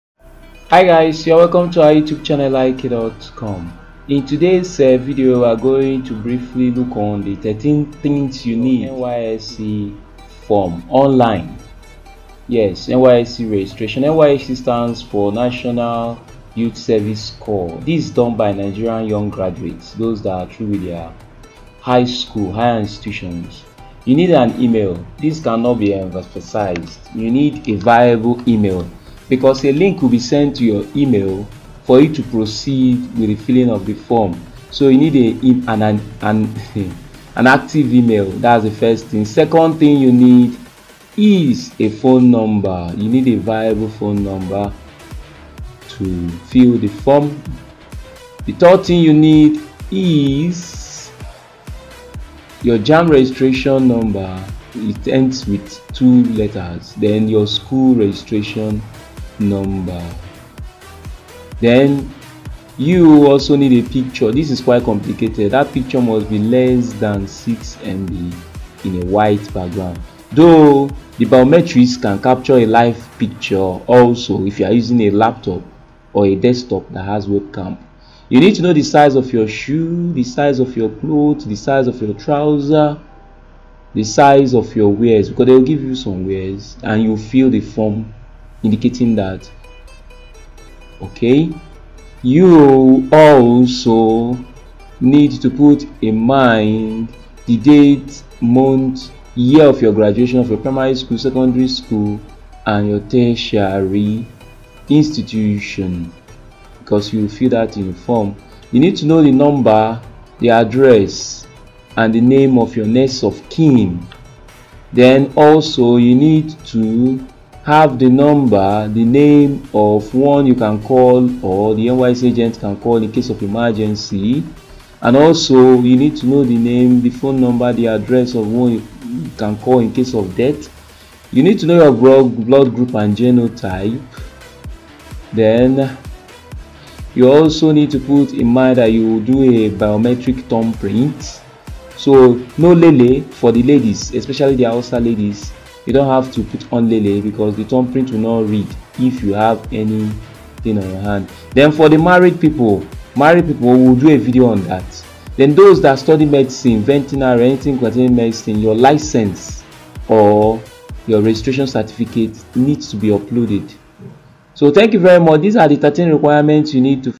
NYSC Registration Form Requirements (2022)
➡️ If You Like our contents, Please⬅️
▬ Hi, Guys!!!  Welcome back again to another tutorial video.
In this Tutorial Video we are going to practically demonstrate step by step:
Today, we are going to practically demonstrate:
➡️NYSC online Registration form Requirement
▬2. 00:20 Email
▬3. 00:40 Phone No.
▬4.
▬5.

                                      NYSC Registration Form Requirements
Here are the basic documents and requirements for registration on NYSC portal:

Your Personal Active Phone Number
Biometric Verification
Passport Photograph
Valid Email Account (Gmail preferably)
Password
JAMB Registration Number
Matriculation Number
First Name
Middle Name
Surname
Gender
Marital Status: If married, provide the following documents newspaper change of name, husband’s valid ID card, marriage certificate, utility bill, and attestation letter from the current employment of husband.
Already Have a Previous Call-up? Yes or No;
Passport (White Background)
Physically Challenged
Nationality
State of Origin
Local Government Area
Present Contact Address
Permanent Address
Town/Village
Next of Kin Data
Institution Attended (Primary School, Secondary School and Higher Institution)
Military Personnel/Paramilitary/National Award Honour Details (if among)
Nigerian Language(s) you speak
Nigerian State(s) visited
Kits ‎Specification (Shirt Size ‎and Trouser Size)
Things to Do (NYSC Registration)
Before Registration

For locally trained graduates, only those whose names appear on the senate/academic board approved result lists submitted by their institutions will have access to register on the NYSC portal.
Locally trained prospective corps members are expected to use correct institution matriculation numbers to register.
During Registration

Fill your details.
Capture Biometric. Note this should not be done by proxy.
Upload clear off white background passport photograph.
Preview your registration form to ensure the details are correct.
Upload your scanned signature or sign using digital signature scanner.
Click on submit.
Keep your login details for subsequent use.
Concessional Posting
Prospective corps members can enjoy concessional posting during registration based on marital or health ground.

Marital Ground

This is for married female prospective corps members who wants to be posted to her husband’s state of domicile.

Such prospective corps members should login to their dashboard and upload the following documents during online registration:

Copies of her marriage certificate
Evidence of change of name and husband’s place of domicile during registration
Husband’s means of identification (Driving License, National ID card etc.)
Health Ground

Prospective corps members with serious health challenge should show their true health status during registration to enable concessional deployment for them.

Medical report from federal or state general hospitals.
Doctor’s details.
Note that those who want to relocate from their current state of deployment to their husband’s state of domicile should apply with the following documents attached.

Marriage certificate.
Newspaper publication for change of name.
Letter from husband employer.
Husband means of identification.
Do I Need to Register on NYSC Portal as a Part-Time Graduate?
Part-time graduates are to register when the NYSC registration portal opens to enable them print their exclusion letter on their dashboard.

                                  ▬▬ Credit and Appreciation To ▬▬
👉▬▬ Video Editing Software KineMaster Pro / ActivePresenter